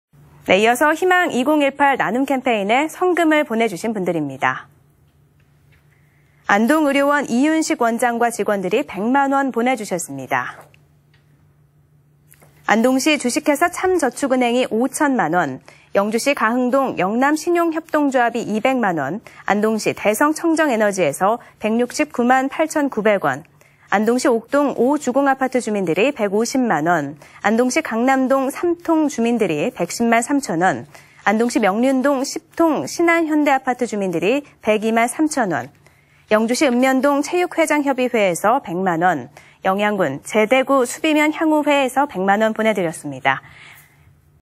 [안동MBC뉴스]이웃돕기 성금[영상+노말]
2018/01/08 16:24:50    작성자 : 보도팀장

다음은 희망 2018, 나눔캠페인에
성금을 보내주신 분들입니다.
◀ＥＮＤ▶

[영상]안동의료원 이윤식 원장과 직원들
100만원 보내 주셨습니다.

안동시 (주)참저축은행 5,000만 원
안동시 남후면 도립노인병원 300만 원
영주시 가흥동 영남신용협동조합 200만 원
영주시 주민자치연합회 200만 원
안동시 대성청정에너지 169만8,900원

안동시 옥동 5주공아파트 주민들 150만 원
안동시 엄마까투리팀 129만 원
안동시 강남동 3통 주민들 110만 3천 원
안동서부초등학교 105만 9,160원
안동시 명륜동 10통 신안현대아파트 주민들
102만3천 원

영주시 읍면동 체육회장협의회 100만 원
영주시 봉현면 두산3리 최병일 100만 원
영양군 재대구수비면향우회 100만 원
안동시 (주)다이나맥스 100만 원
안동시 옥동 (주)대안 100만 원

안동시 서후면 장안축사 김성자대표 100만 원
안동시 (주)샘터정보기술 100만 원
안동시 강남동 영남레미콘(주) 100만 원
안동시 남선면 대흥환경산업 100만 원
안동시 동맥회 100만 원

안동시 수하동 동방환경 100만 원
안동시 강남동 현진1차아파트 주민들 100만 원
안동시 상공회의소 100만 원
안동시 남후면 삼원기업 100만 원
안동시 도산면 가송리 남병태씨 100만 원
보내주셨습니다.

성금을 보내 주신 분들께 감사드립니다.